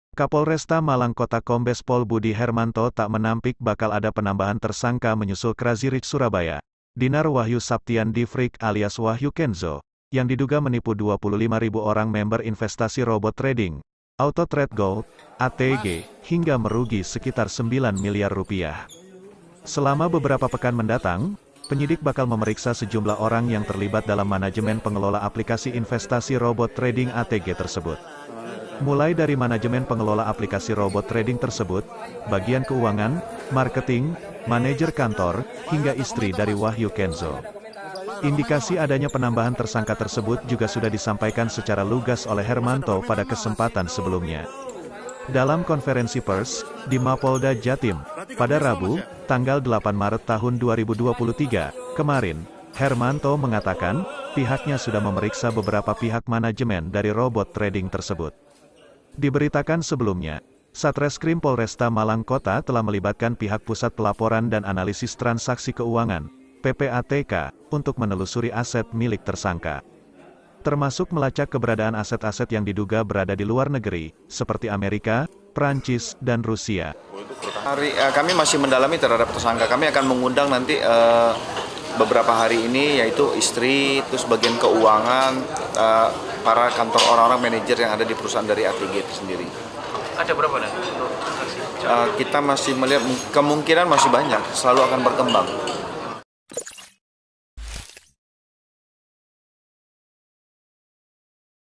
Robot Trading ATG Bakal Seret Tersangka Baru? Istri Crazy Rich Surabaya Wahyu Kenzo Bakal Diperiksa
video & reporter : Luhur Pambudi / zaq

TRIBUNJATIM.COM, SURABAYA-Kapolresta Malang Kota Kombes Pol Budi Hermanto tak menampik bakal ada penambahan tersangka menyusul Crazy Rich Surabaya, Dinar Wahyu Saptian Dyfrig alias Wahyu Kenzo, yang diduga menipu 25 ribu orang member investasi Robot Trading, Auto Trade Gold (ATG), hingga merugi sekitar sembilan miliar rupiah.

Selama beberapa pekan mendatang, penyidik bakal memeriksa sejumlah orang yang terlibat dalam manajemen pengelola aplikasi investasi robot trading ATG tersebut.

Mulai dari manajemen pengelola aplikasi robot trading tersebut, bagian keuangan, marketing, manejer kantor, hingga istri dari Wahyu Kenzo.